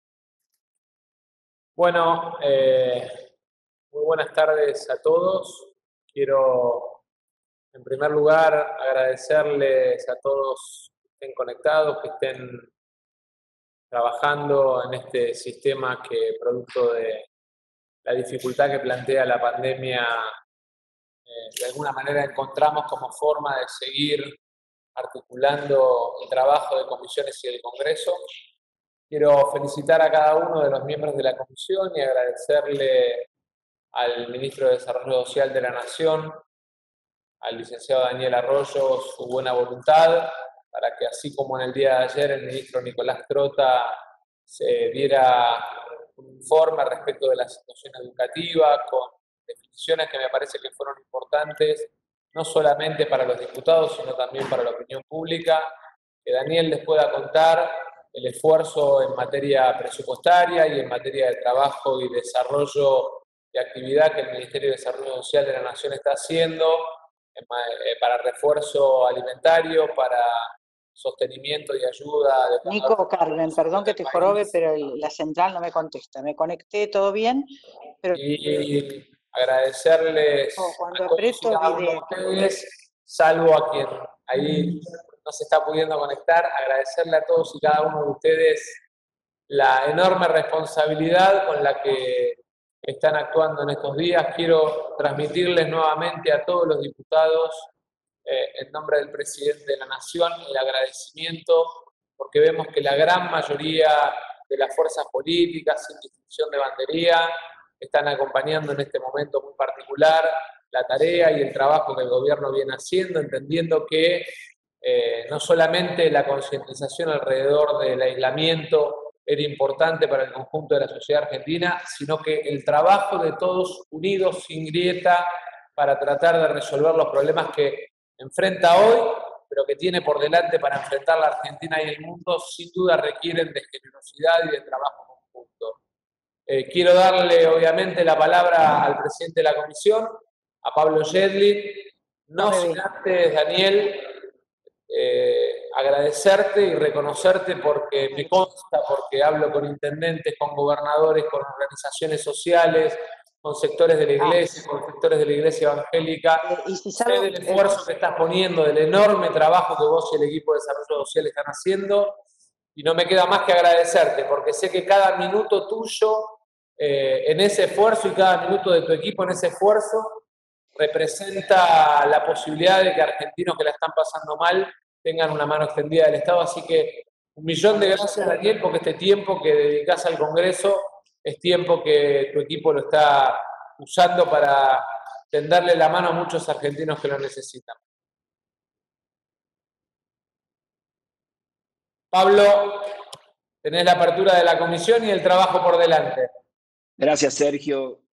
Diputado Massa, Sergio - Comisión Acción Social y Salud Pública - 01-04-2020
Reunión por videoconferencia de la Comisión de Acción Social y Salud Pública con la participación  de los ministros Daniel Arroyo (Ministro de Desarrollo Social) y Ginés Gonzalez Garcia (Ministro de Salud).